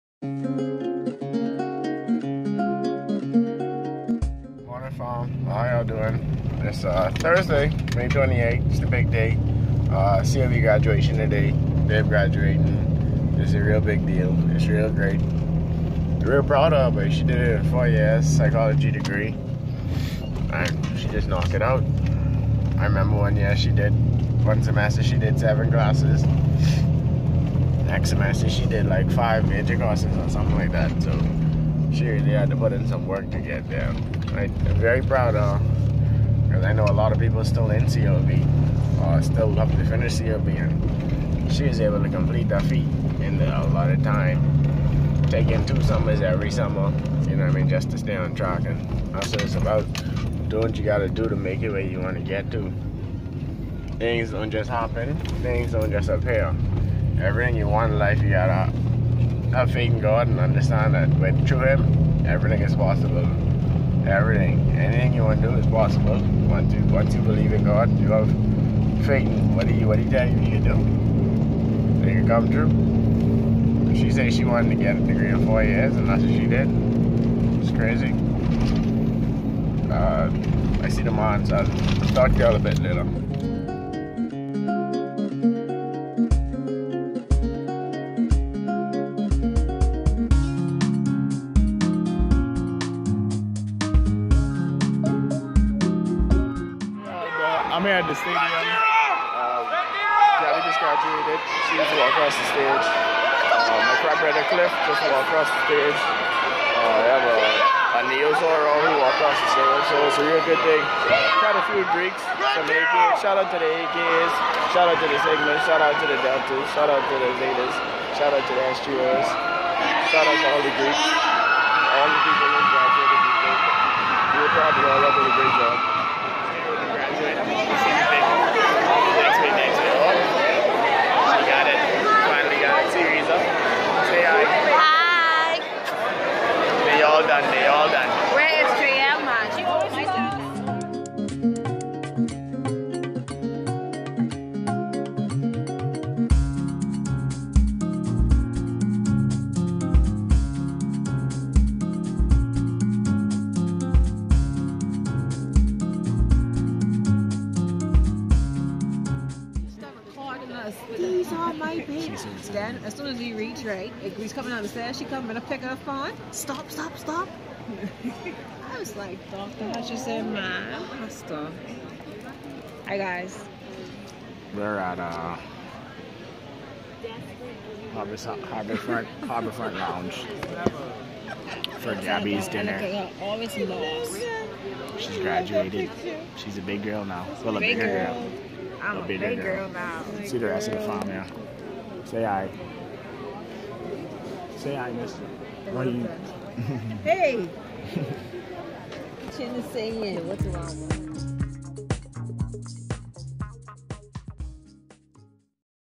Babe Graduation Day | (vlog 4)ParrBoy Life
Class of 2015 College of The Bahamas ceremony

Don't forget to hit me up on Twitter/Instagram/Soundcloud @MondoPb3

Film and Edit:
Camera: Samsung Note 4
Editing: KindMaster
Shooting: MondoPb3